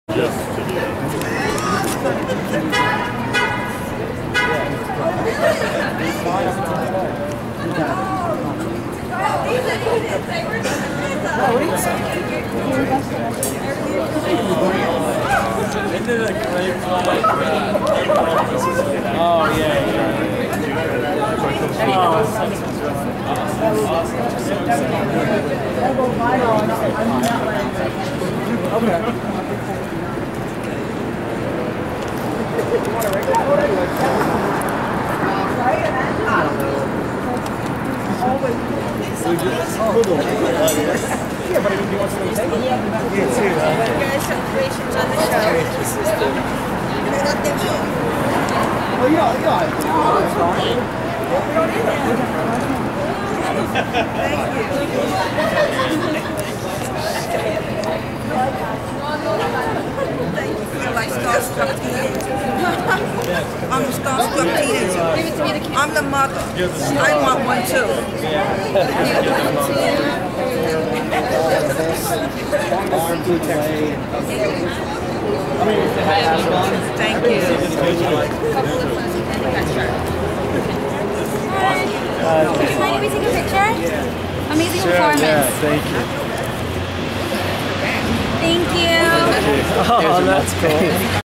Mark Ruffalo Greets Fans Stage Door The Price Broadway
Mark Ruffalo greets fans at the stage door of Arthur Miller's "The Price." The Avengers star along with Tony Shalhoub are the nicest people in the cast by far posing for pictures and signing autographs.